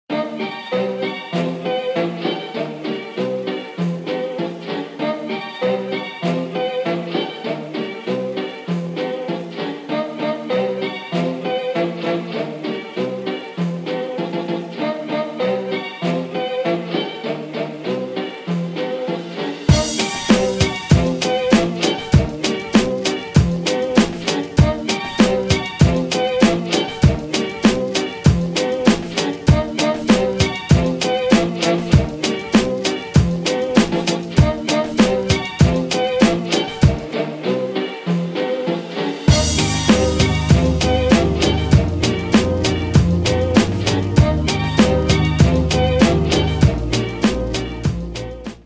Bomboniera Scatolina, costruiscila in pochi semplici passi con il tutorial Lieti Eventi AIL
Pochi semplici passi per montare le bomboniere dell'AIL!